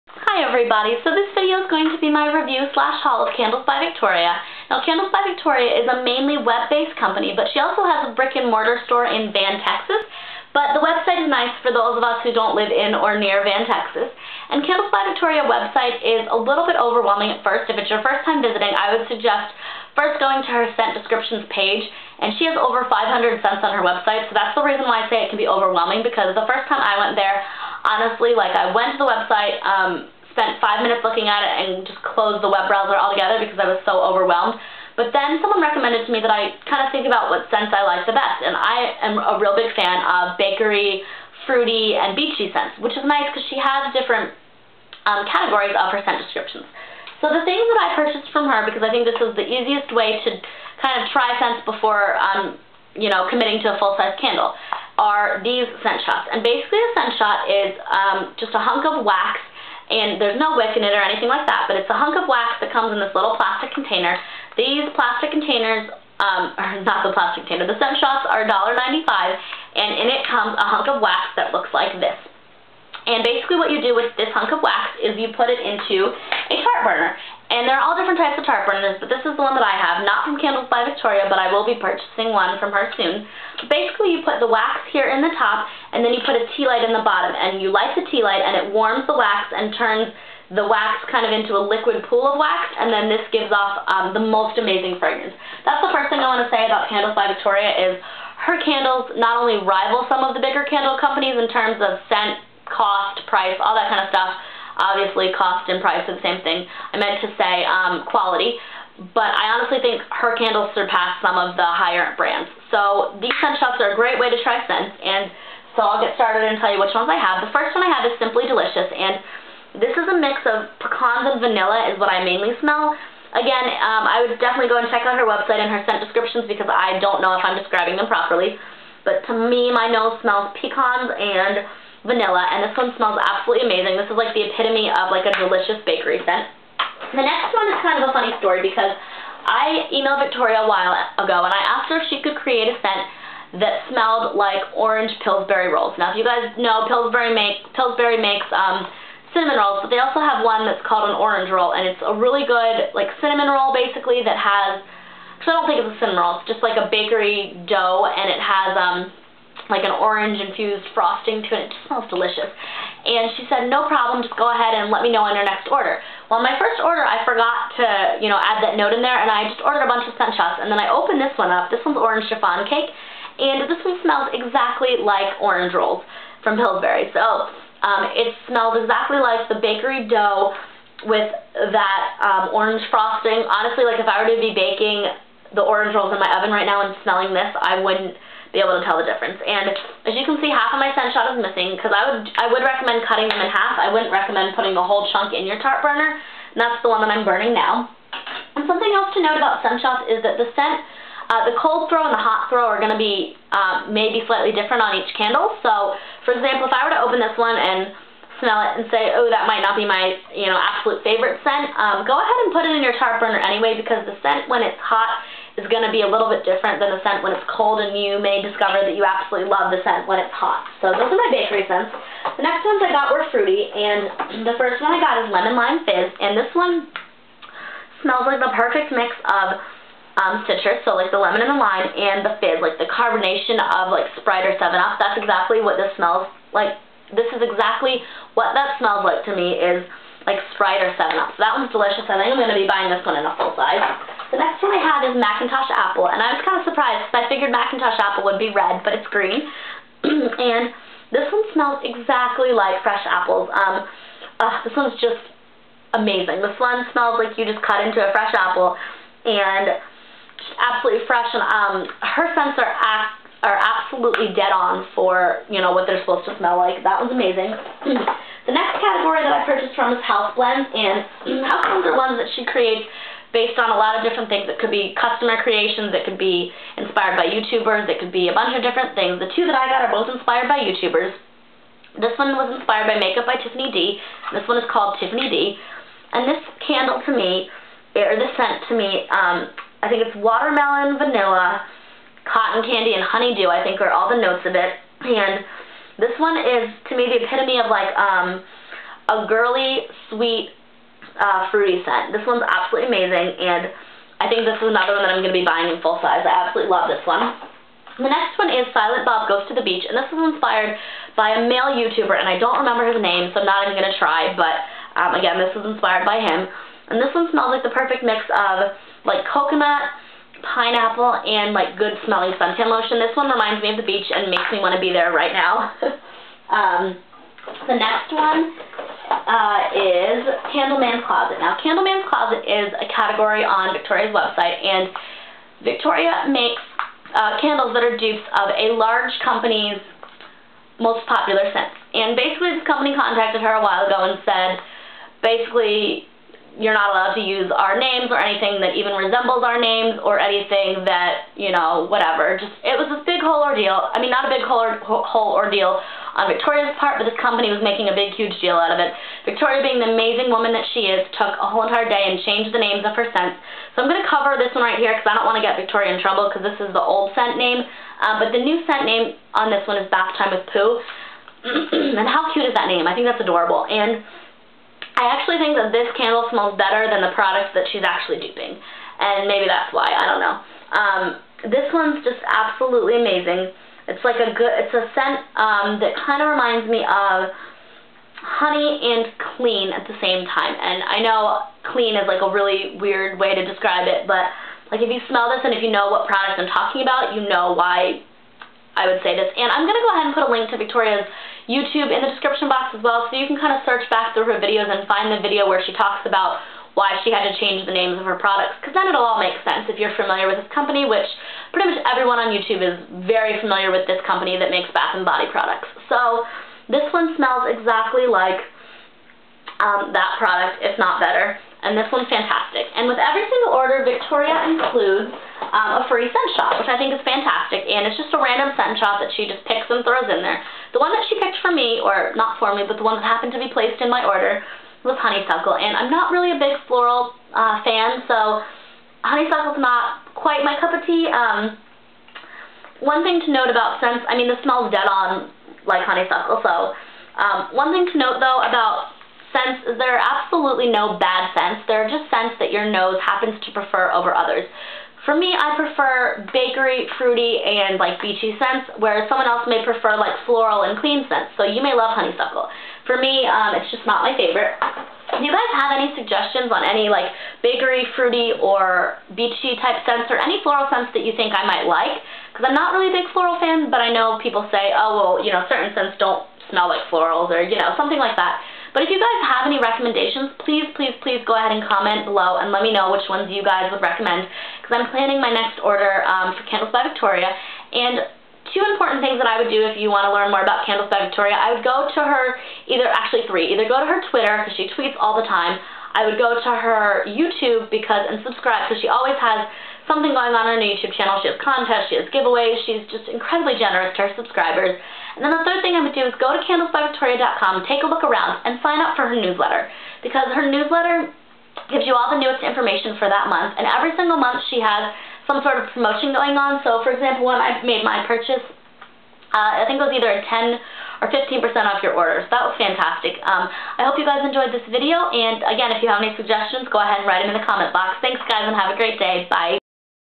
Candles by Victoria Haul/Review
Products Mentioned:
All Scent Shots -$1.95

Simply Delicious
Orange Chiffon Cake
Lemon Lime Fizz
Macintosh Apple
Georgia Peach (Forgot to show this one!)
TiffanyD
Silent Bob Goes to the Beach
Bathtime with Pooh
Honeysuckle